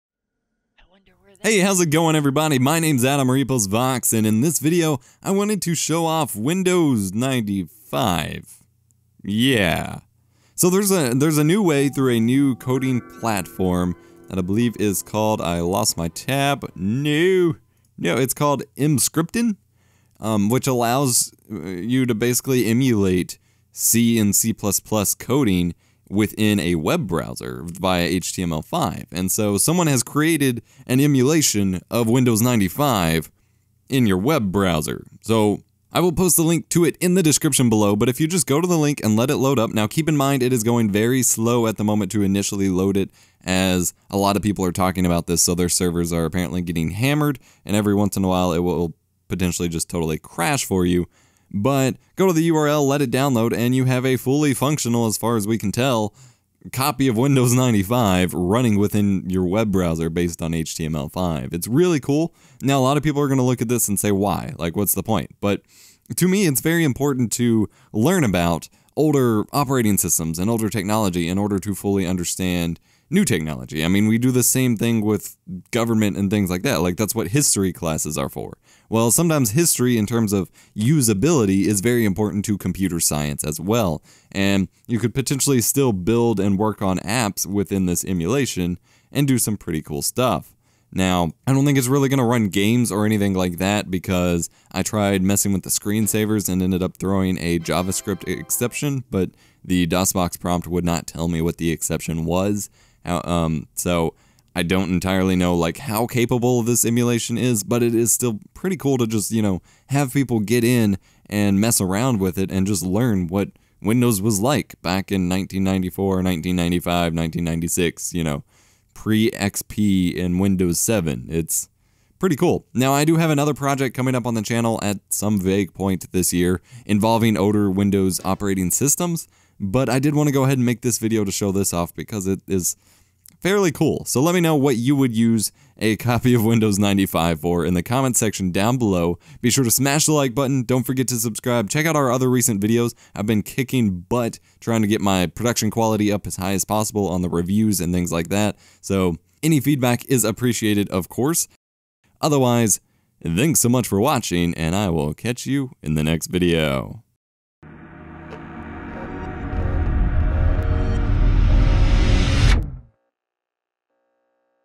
WINDOWS 95 IN YOUR BROWSER?!
Yes! You can use a full copy of Windows 95 within your web browser! Technology is scary at times... Wow!